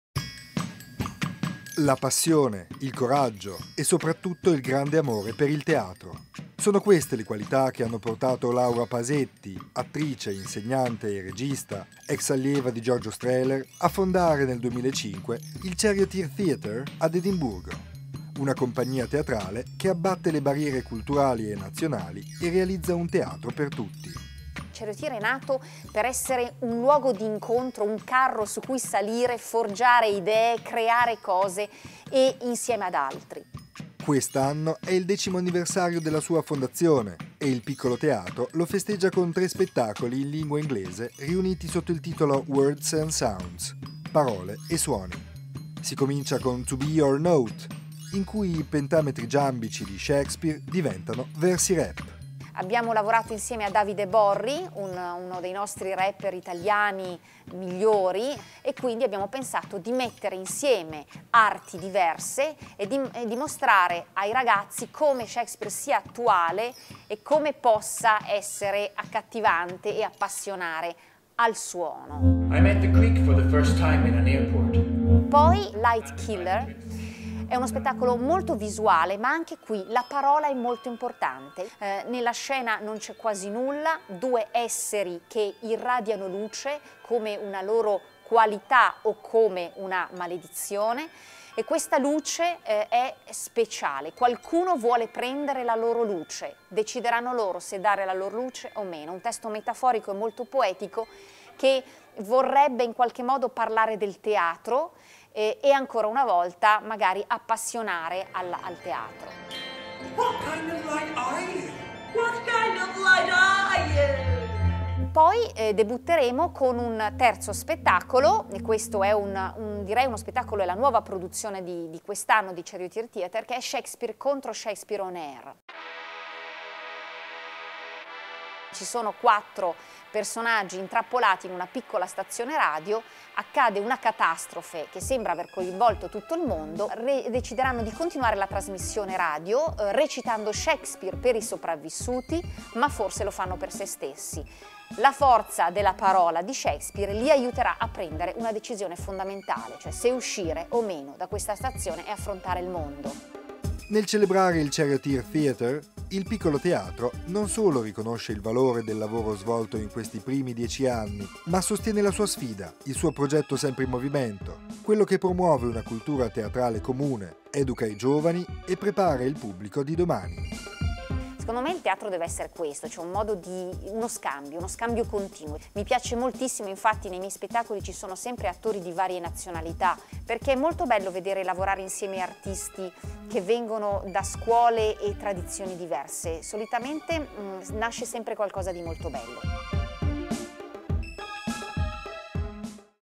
Wordsandsounds - Laura Pasetti | Piccolo Teatro di Milano
All’ottava stagione sulle scene del Piccolo, la compagnia scozzese Charioteer Theatre, fondata e diretta da Laura Pasetti, in occasione del quattrocentesimo anniversario della morte di Shakespeare, è non soltanto ospite privilegiato ma anche coproduttore con lo speciale progetto WORDSANDSOUNDS: SHAKESPEARE 2.016 che comprende SonsOfGod - progetto Coriolano, The Merry Wives of William e Breakfast with Shakespeare.

Piccolo Teatro Studio Melato
dal 13 al 29 aprile 2016